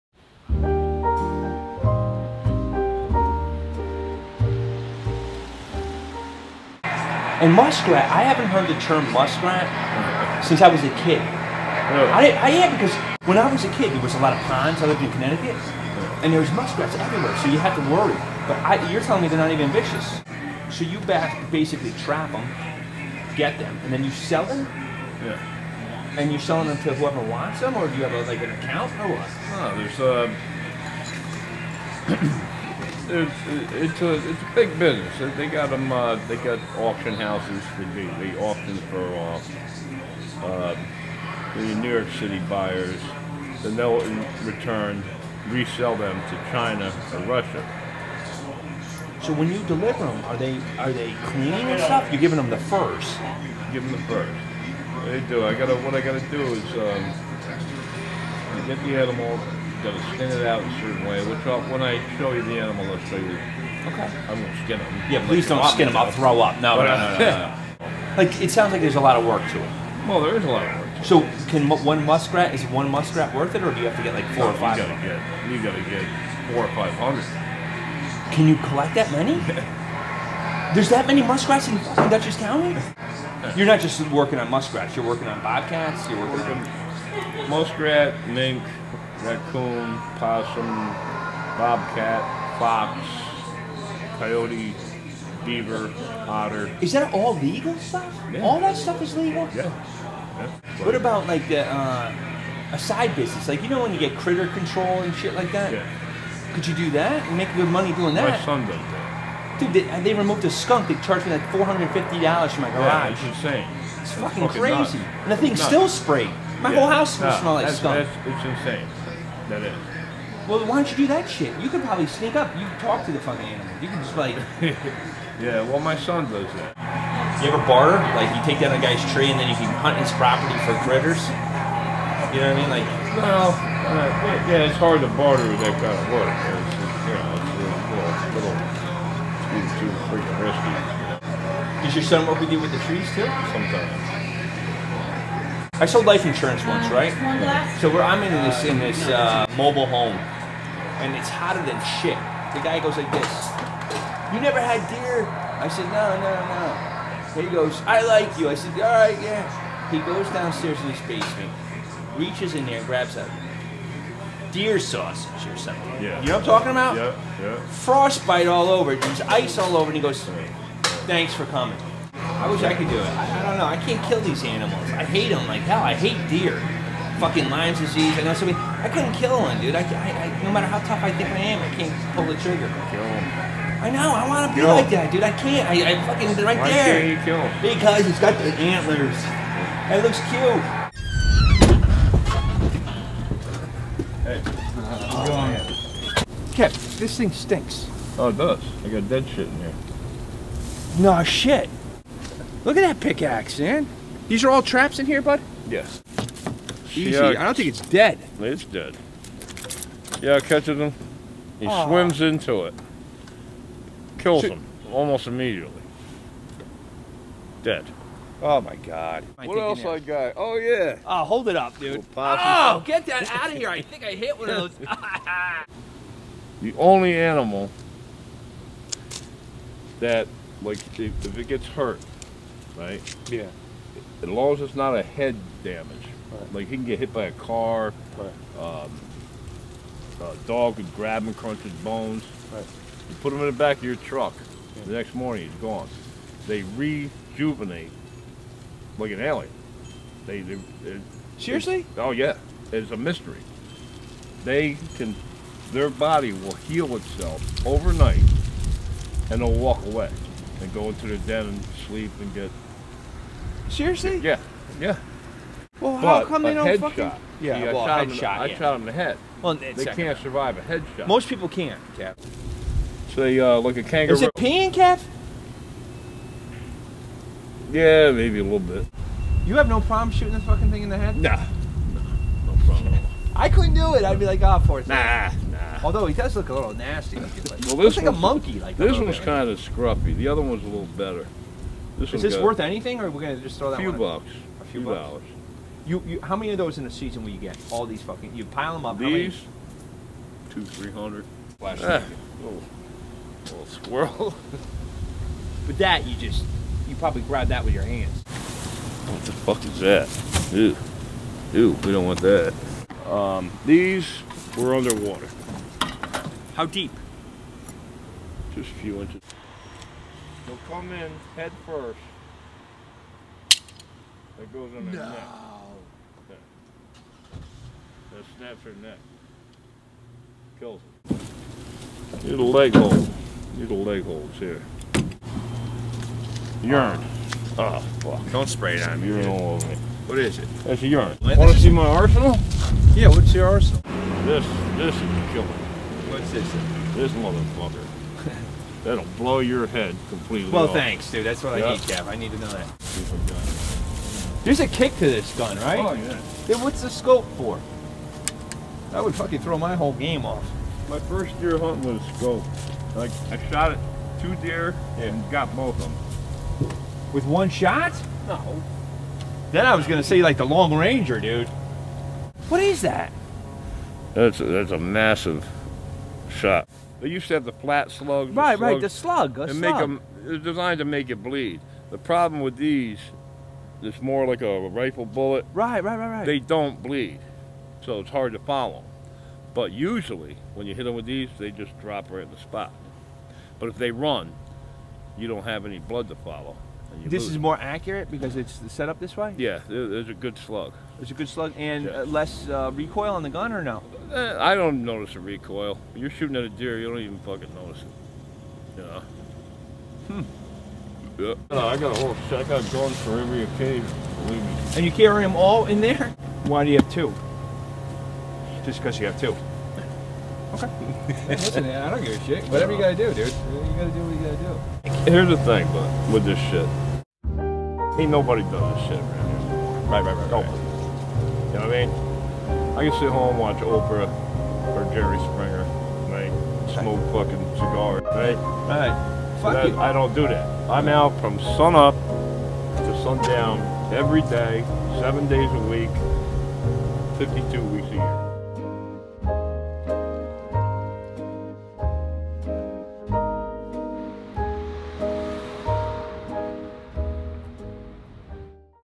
Kevin
Adam Izzo drops by "Joe's Bar and Grill" in Beacon, NY for a slice of life.

Created by: Adam Izzo and Mark Alderson
Directed and Shot by: Mark Alderson